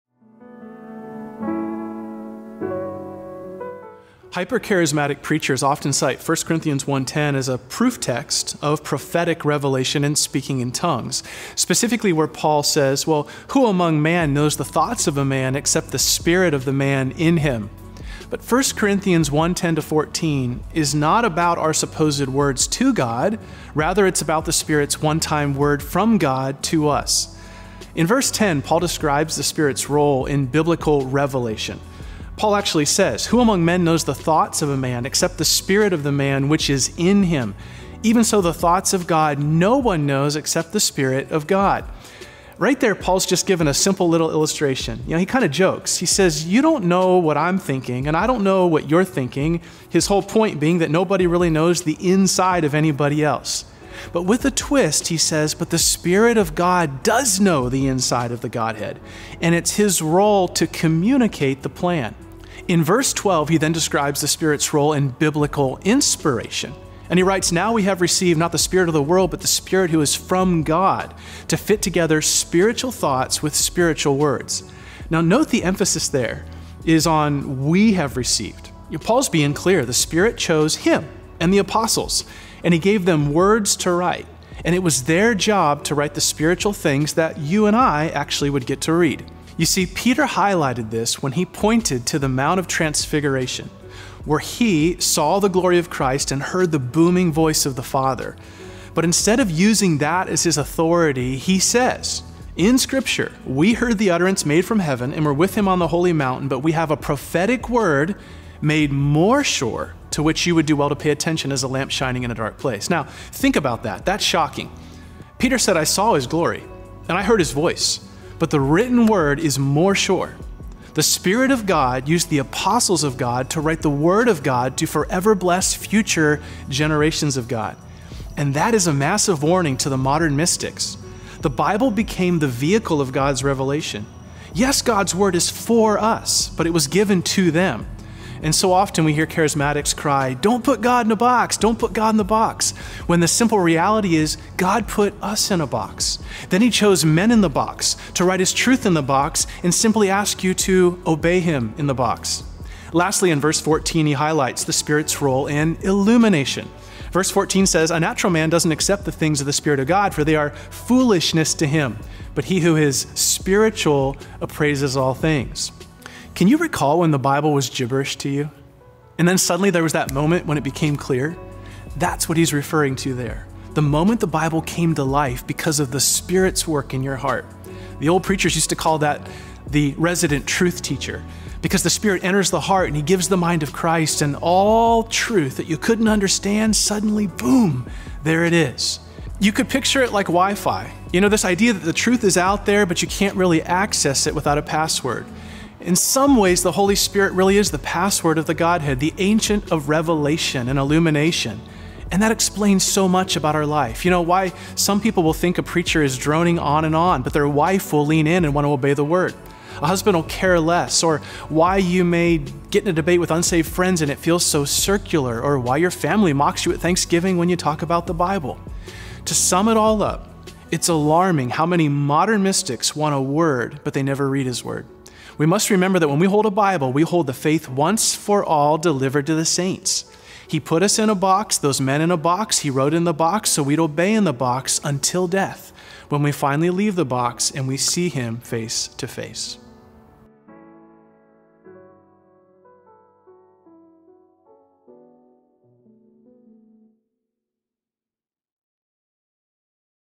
Cutting Through The Charismatic Chaos | Anthony Wood
God’s Word is sufficient.  In this video, Anthony Wood explains why believers should not be seeking a “word from the Lord” outside of Scripture or listening to those who claim they receive direct revelation from God today.